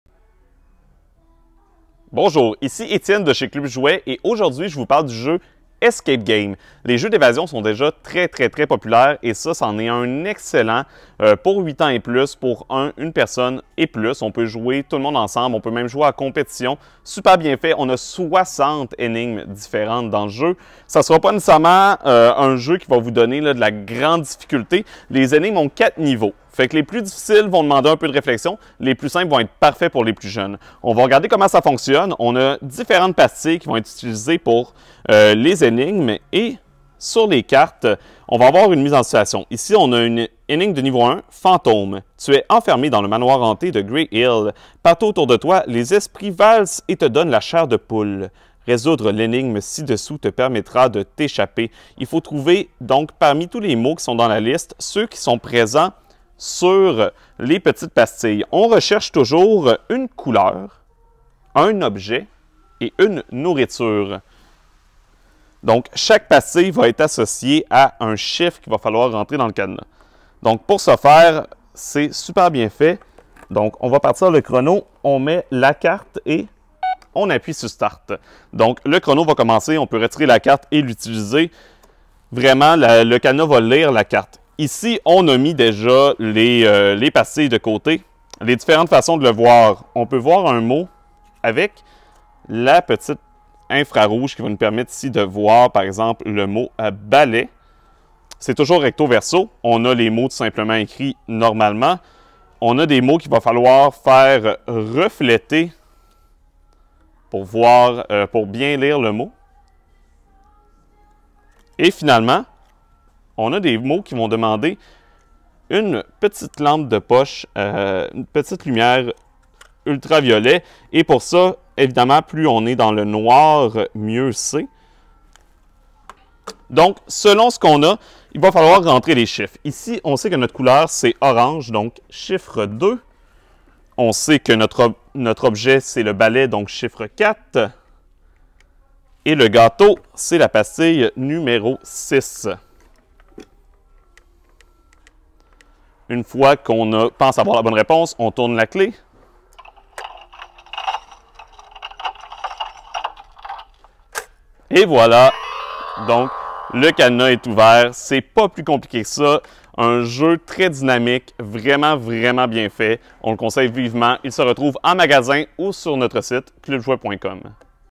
Comment jouer à Escape Game Le Cadenas Électronique de Dujardin
Étienne vous explique comment jouer à Escape Game: le cadenas électronique.

Vous êtes enfermés dans une pièce verrouillée par un cadenas. Votre mission : résoudre l’énigme, retrouver les indices pour ouvrir le cadenas et vous échapper avant qu’il ne soit trop tard !

Ce jeu contient :

1 cadenas électronique
30 cartes énigmes recto-verso
20 jetons indices recto-verso
1 lampe de lumière noire
1 miroir
1 filtre rouge
1 livret de règle du jeu
Piles non incluses (3x AA pour le cadenas, 3x AAA pour la lampe)

8+ ans / 1+ joueur / 15 minutes